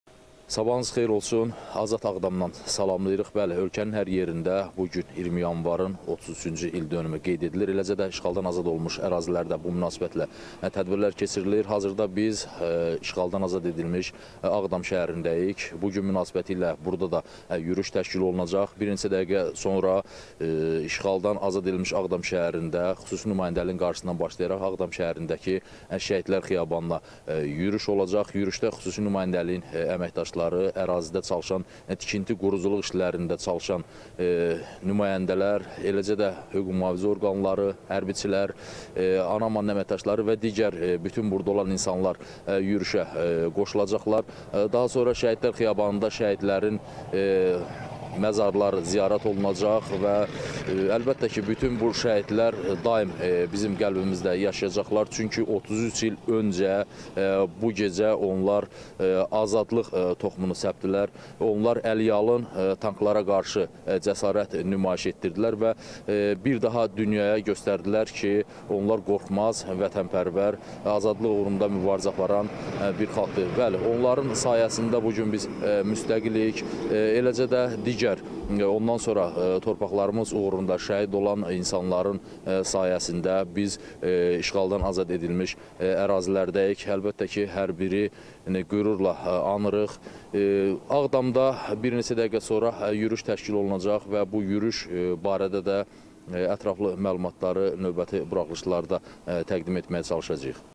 Azad Ağdam şəhərində yürüş keçiriləcək - Şəhidlərin xatirəsi yad olunur
AZAD OLUNMUŞ ƏRAZİLƏRDƏ ŞƏHİDLƏRİN XATİRƏSİ YAD OLUNUR